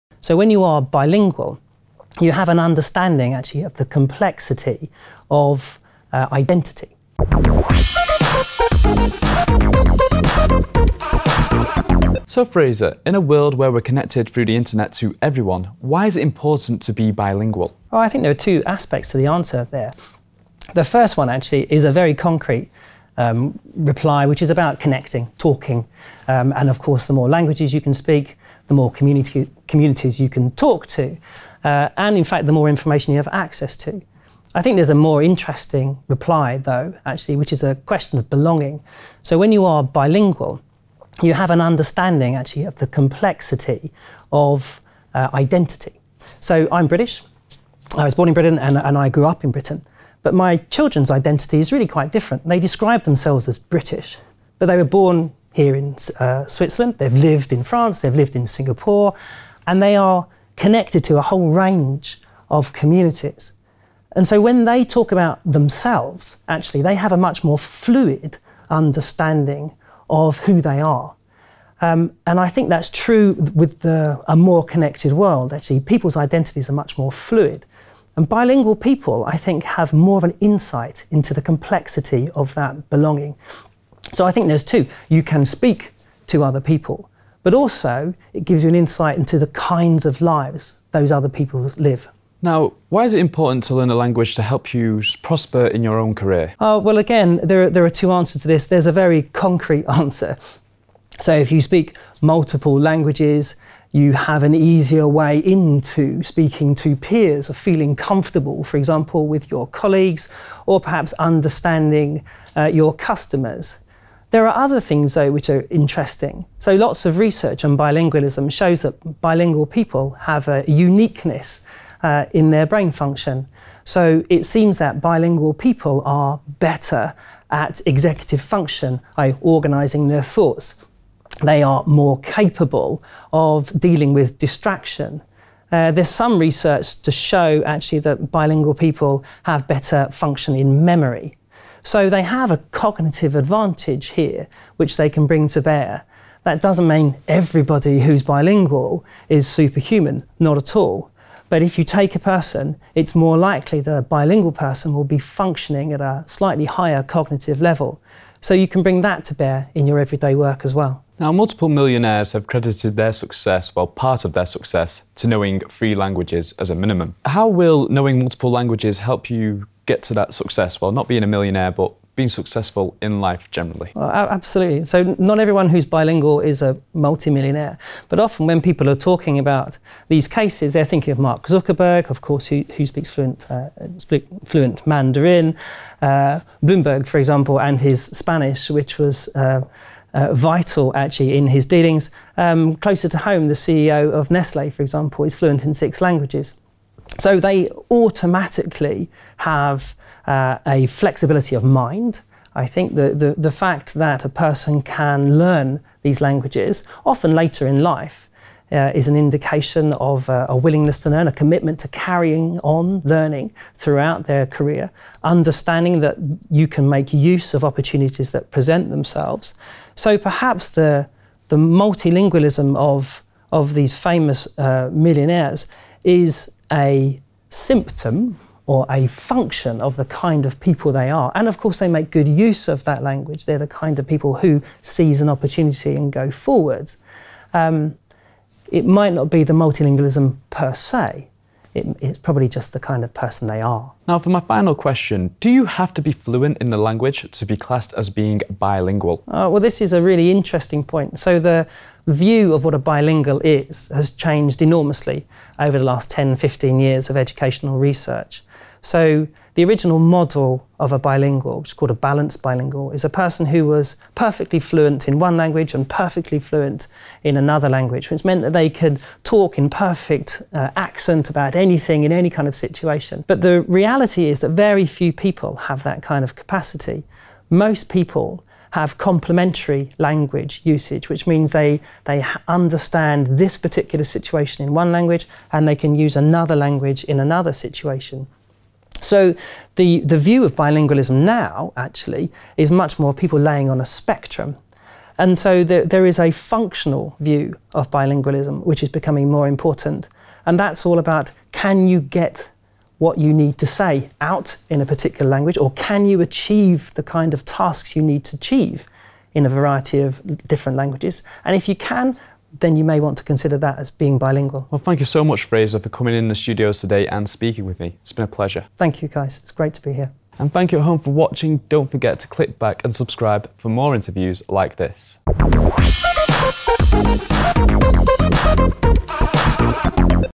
Being Bilingual Boosting Business
How can being a multilingual benefit you in an international business world? Dr Frazer Cairns, The International School of Lausanne